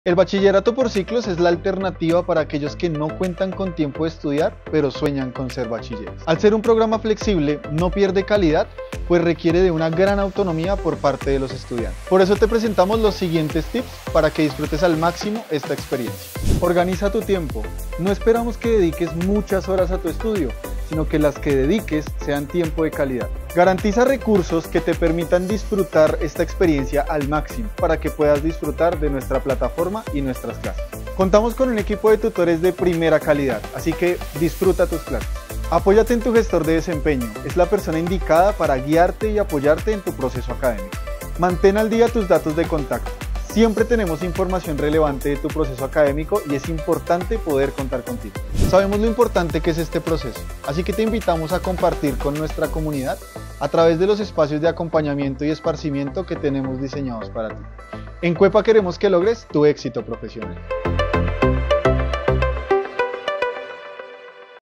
Termina tu bachillerato, por ciclos 🎓
En este video, te revelamos estrategias efectivas para optimizar tu aprendizaje y alcanzar el éxito académico. 🦾 No te pierdas esta oportunidad de ser bachiller. 📚✨